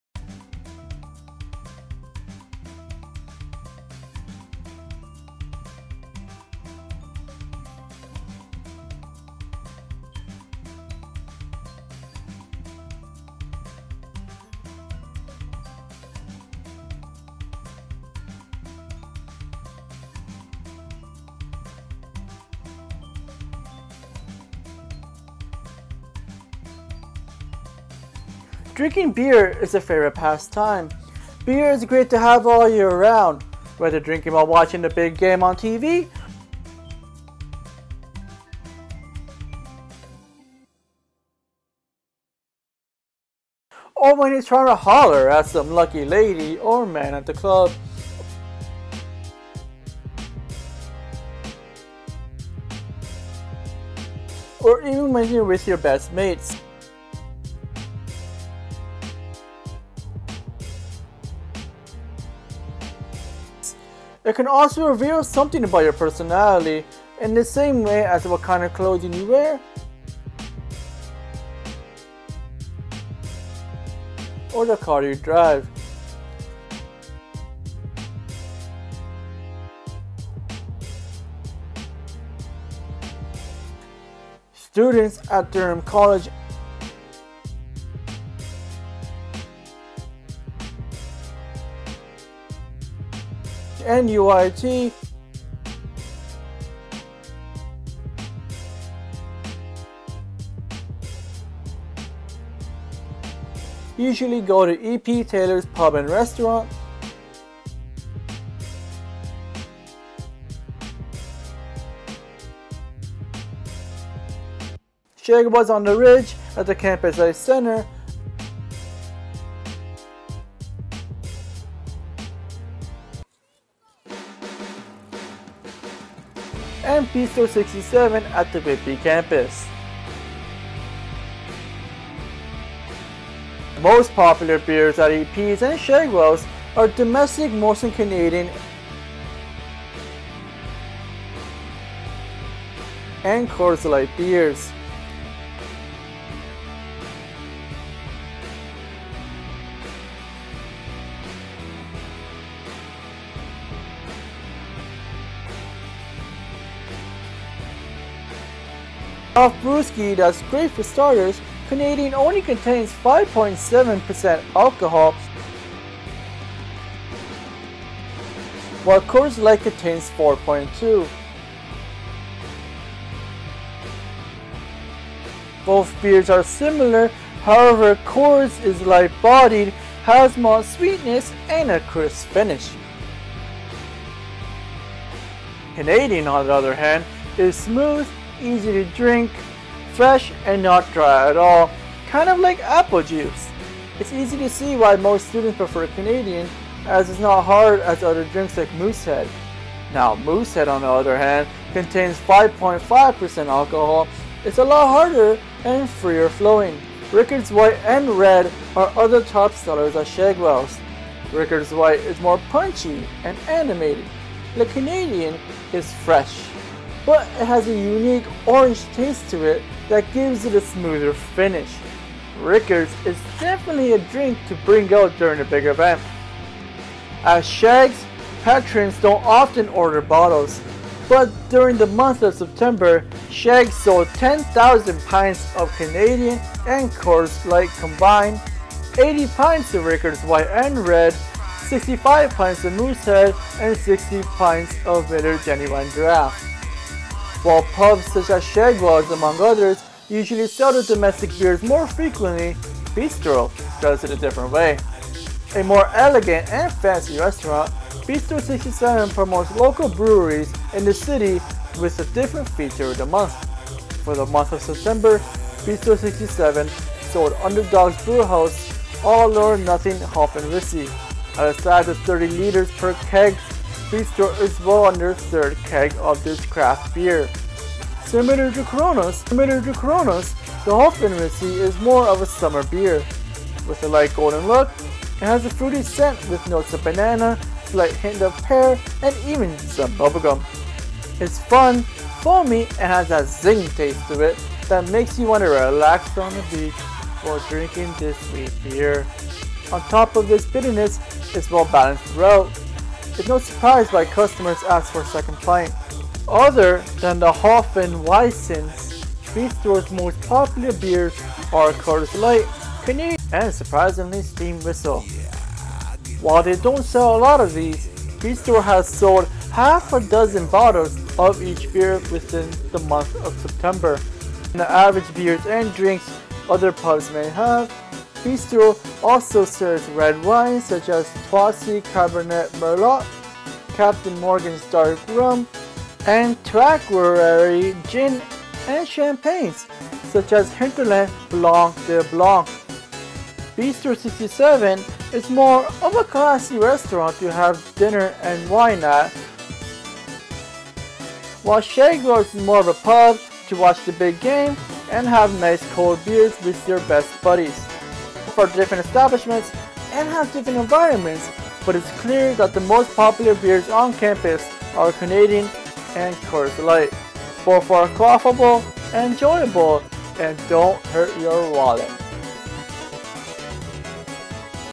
Beer Multimedia Video
A multimedia video on the best beers on campus at Durham College and UOIT.